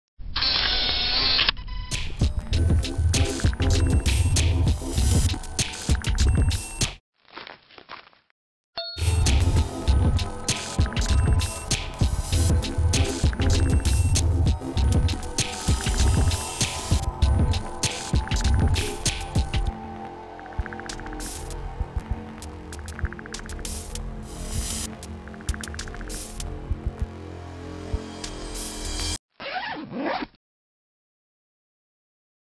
PLUG-IN : the safest risk _ video commercial 3
With plug-in, taking care of your DSLR and changing lens while photography is not a hassle anymore.
With unique design features, It can be placed in any type of stylish bag to be used as a camera bag.
For lens-changing, It can be fixed on your waist to protect your extra lenses. So photographers with multiple lenses can use it as lens-changing -harness in waist-bag mode, with ultimate safety to lenses unlike other lens-changing-harness which keeps lenses exposed for possible damage.
It's body is made of multi-layers of tough fabric providing superior strength & water resistance and its metal frame structure guards the camera from all kinds of damages. Inner bean bag is the place where your precious lens can be kept safely.
Plug-in is tested and proven with 100Kg weight, with a camera inside. So let Plug-in take care of your beloved camera and have a carefree shooting experience!